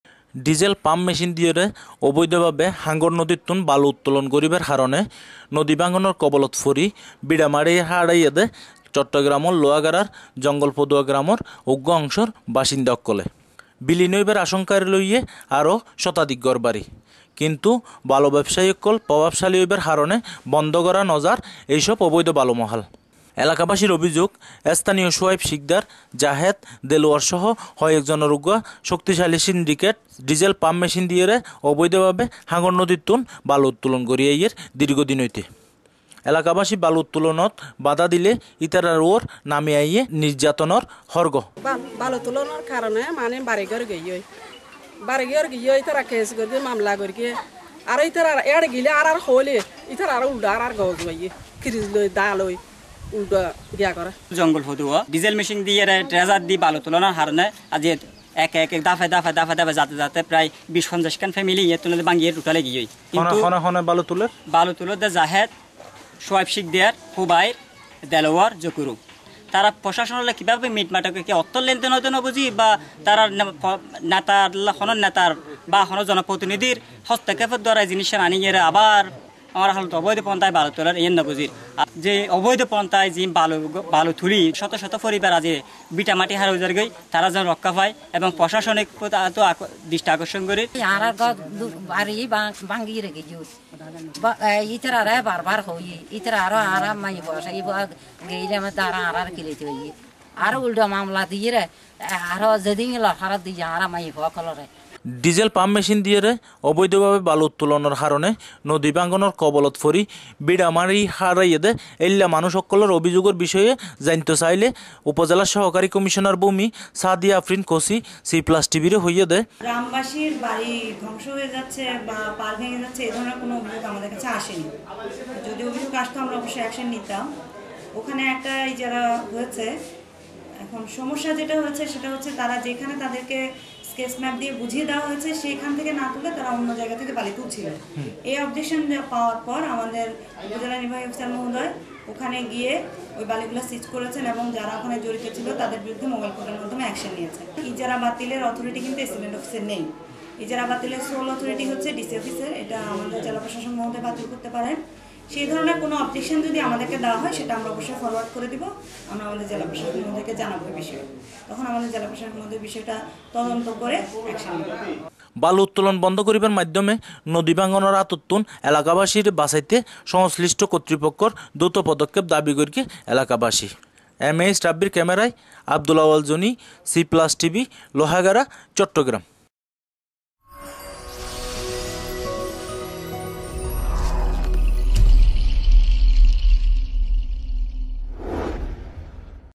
অবৈধভাবে বালু উত্তোলনের কারণে নদী ভাঙ্গনের কবলে লোহাগাড়ার জঙ্গল পদুয়া গ্রামের বাসিন্দারা
ডিজেল পাম্প মেশিন দিয়ে অবৈধভাবে হাঙ্গর নদী থেকে বালু উত্তোলনের কারণে নদী ভাঙ্গনের কবলে পড়ে ভিটেমাটি হারিয়েছে চট্টগ্রামের লোহাগাড়ার জঙ্গল পদুয়া গ্রামের একটি অংশের বাসিন্দারা, বিলিন হওয়ার আশংকায় রয়েছে আরো শতাধিক বাড়িঘর, কিন্তু বালু ব্যাবসায়িরা প্রভাবশালী হওয়ায় বন্ধ করা যাচ্ছেনা এসব অবৈধ বালুমহাল।
এলাকাবাসীর অভিযোগ স্থানীয় শোয়াইব সিকদার, জাহেদ, দেলোয়ার সহ কয়েকজনের একটি শক্তিশালী সিন্ডিকেট ডিজেল পাম্প মেশিন দিয়ে অবৈধভাবে হাঙ্গর নদী থেকে বালু উত্তোলন করে আসছে, এলাকাবাসী বালু উত্তোলনে বাধা দিলে তাদের উপর নেমে আসে নির্যাতনের ঘড়গ।
ডিজেল পাম্প মেশিন দিয়ে অবৈধভাবে বালু উত্তোলনের কারণে নদী ভাঙ্গনের কবলে পড়ে ভিটেমাটি হারানো লোকদের অভিযোগের বিষয়ে জানতে চাইলে উপজেলা সহকারী কমিশনার (ভুমি) সাদিয়া আফরিন কচি বলেন, তিনি এখনো লিখিত কোন অভিযোগ পাননি পেলে ব্যবস্থা নিবেন এবং সংশ্লিষ্ট বালুমহালের ইজারা বাতিলের জন্য চট্টগ্রাম জেলা প্রশাসককে জানাবেন।
বালু উত্তোলন বন্ধ করার মাধ্যমে নদীভাঙ্গনের হাত থেকে এলাকাবাসীকে বাচাতে সংশ্লিষ্ট কতৃপক্ষের দ্রুত পদক্ষেপ দাবী করছে এলাকাবাসী।